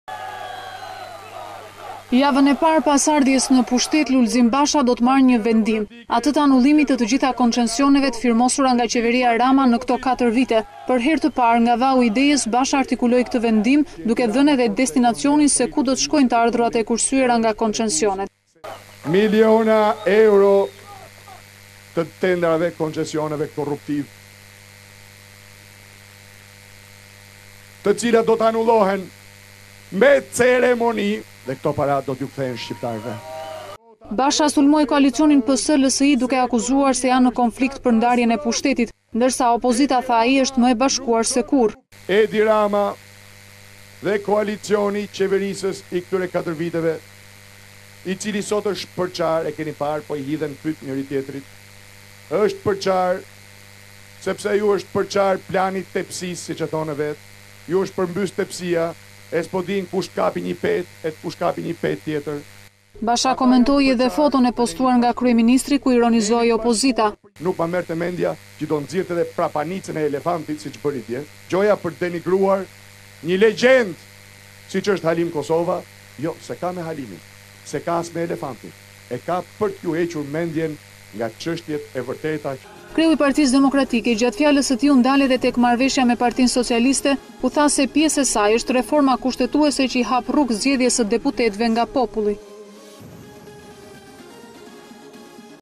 Fushata zgjedhore - Basha premton: Do anuloj koncesionet e Ramës me ceremoni
Partia Demokratike, sapo të vijë në pushtet, do të anulojë të gjitha koncesionet korruptive të qeverisë Rama.
Gjatë takimit më mbështetësit në Vaun e Dejës, kryetari i PD-së, Lulzim Basha artikuloi këtë vendim duke treguar dhe se ku do të shkojnë të ardhurat.
Lulzim Basha: Miliona euro të tenderave e koncesioneve korruptivë të cilat do anulohen me ceremoni dhe këto para do u kthehen shqiptarëve”